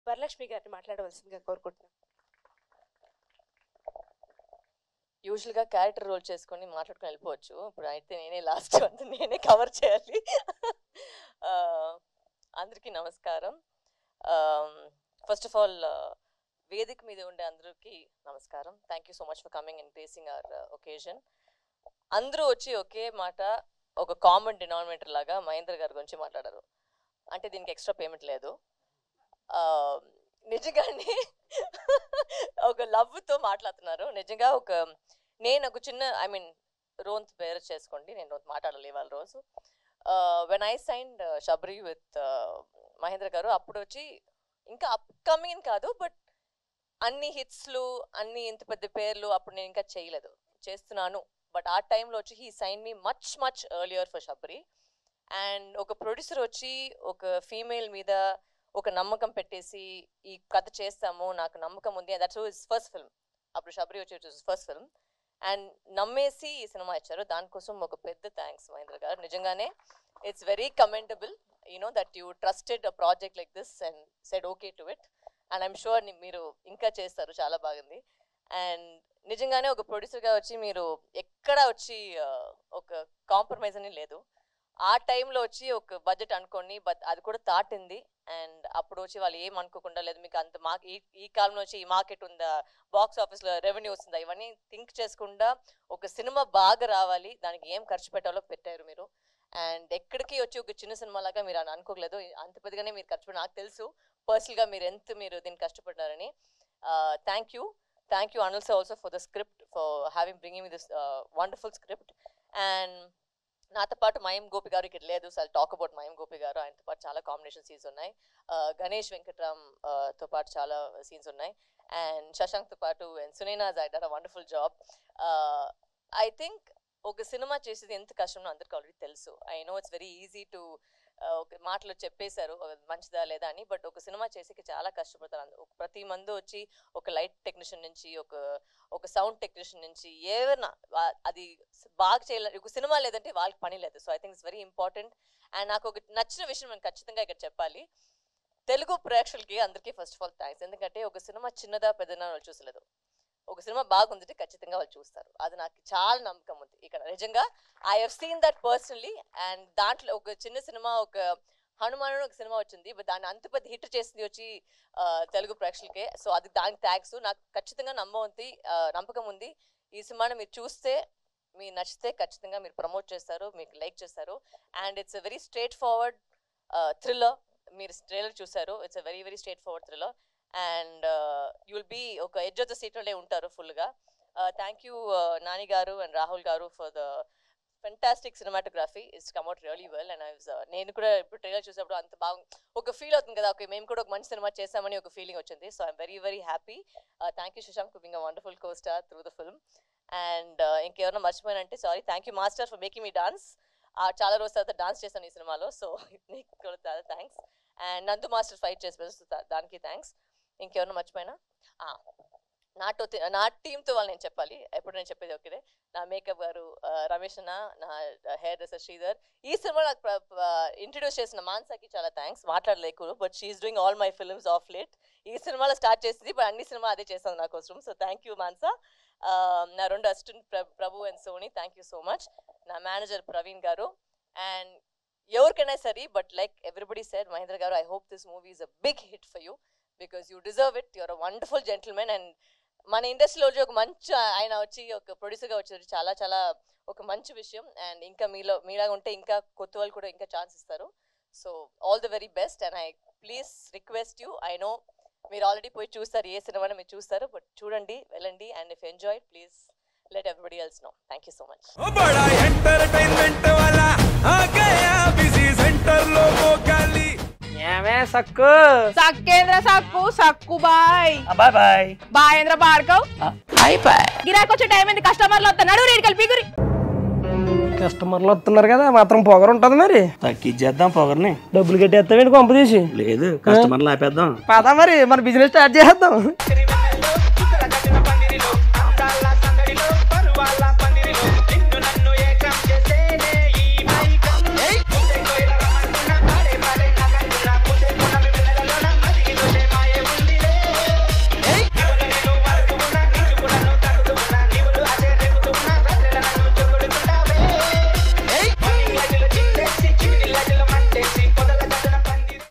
Varalaxmi Sarath Kumar Speech @ Sabari Trailer Release Press Meet
Watch :Varalaxmi Sarath Kumar Speech @ Sabari Trailer Release Press Meet | @HitTVTalkies